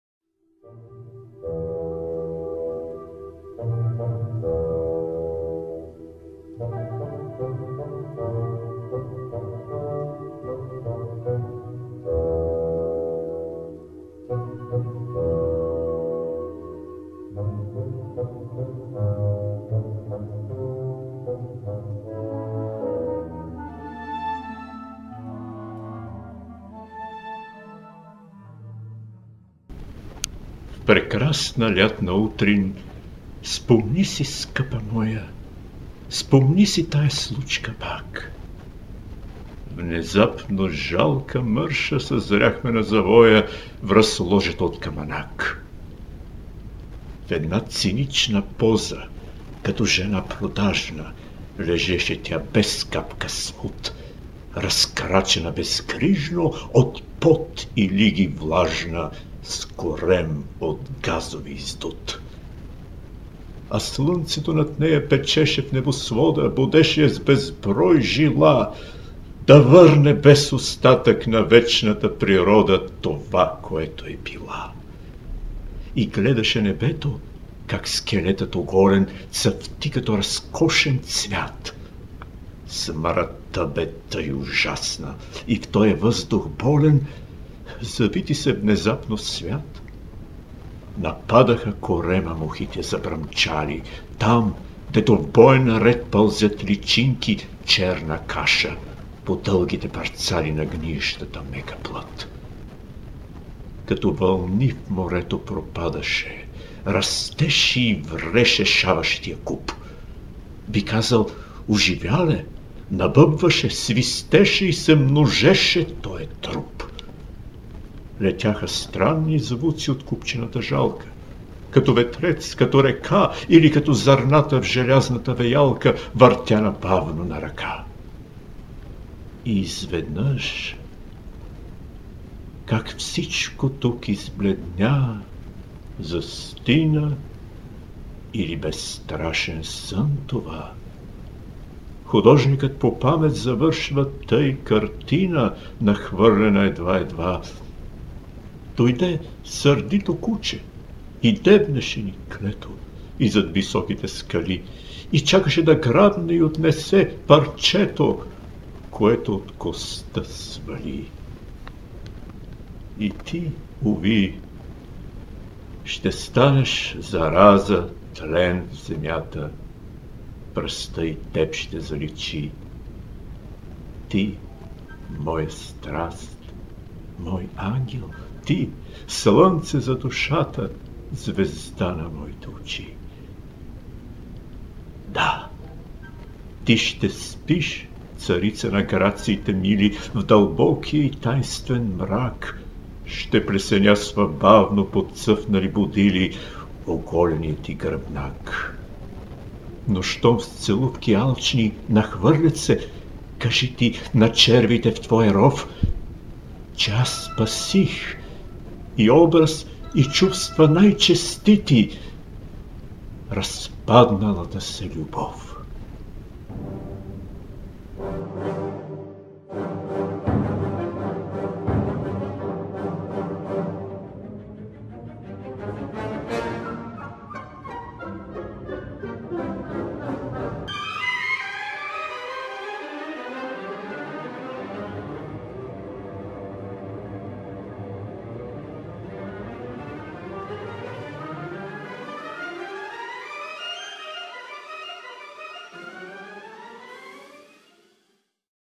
Шарл Бодлер - "Мърша", рец. Богдан Дуков
Шарл Пиер Бодлер (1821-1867) е френски поет, критик и преводач, една от най-влиятелните и противоречиви фигури във френския културен живот на 19 век. През 1857 Бодлер публикува своята първа и най-известна стихосбирка „Цветя на злото“ („Les fleurs du mal“). Тя е издадена от неговия приятел Огюст Пуле Маласис, който наследява печатница в Алансон. Стихотворенията са високо оценени от малък кръг читатели, но сюжетът им предизвиква недоволството на критиката и широката публика. Основните теми за секса и смъртта са смятани за скандални и книгата се превръща в нарицателно за неблагоприличие. Бодлер, издателят му и печатарят му са съдени за нарушаване на обществения морал. Стихосбирката е преиздадена през 1861 със значителни допълнения./ Charles Pierre Baudelaire (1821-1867) war ein Französisch Dichter, Kritiker und Übersetzer, einer der einflussreichsten und umstrittensten Figuren in Französisch Kulturleben des 19. Jahrhunderts. Im Jahr 1857 veröffentlichte Baudelaire seine erste und berühmteste Sammlung von Gedichten "Blumen des Bösen" ( "Les fleurs du mal"). Es wurde von seinem Freund Auguste Poulet Malasis ausgestellt, die Druck auf Alanson erbt. Die Gedichte sind stark von einem kleinen Kreis von Lesern geschätzt, aber ihre Ursachen Unzufriedenheit der Kritiker und der Öffentlichkeit darzustellen. Die Hauptthemen Sex und Tod wurden als skandalös und das Buch wurde zu einem Synonym für indecorum. Baudelaire, seinen Verleger und Drucker haben wegen Verletzung der öffentlichen Sittlichkeit verklagte ihn. Poetry Buch wurde 1861 mit signifikanten Ergänzungen abgedruckt. „ Aas“ – Rezitiert und Inszeniert von Bogdan Dukov, 2016/.